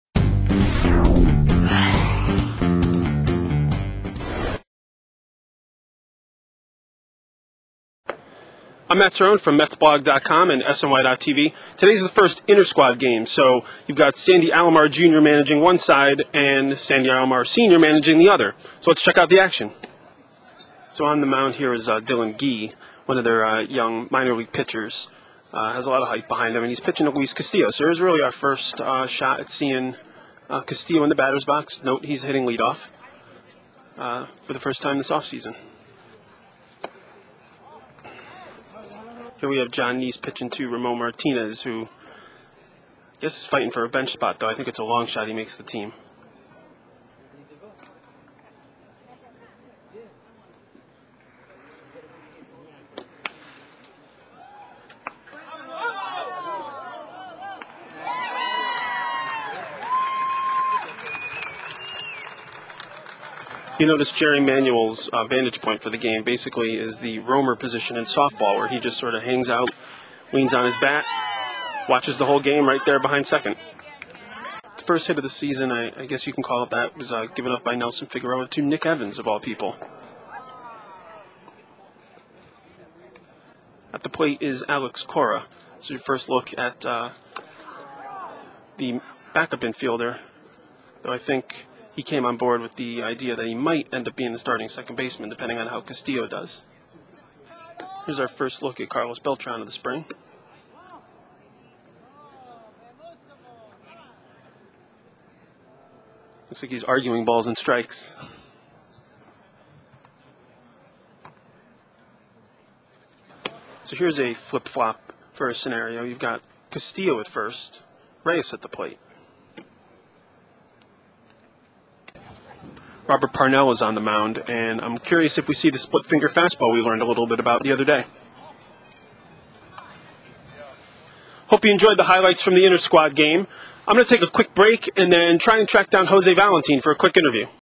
SNY.tv - Mets Intra-Squad Game
MetsBlog's Matthew Cerrone's video diary for SNY featuring today's Mets intra-squad game.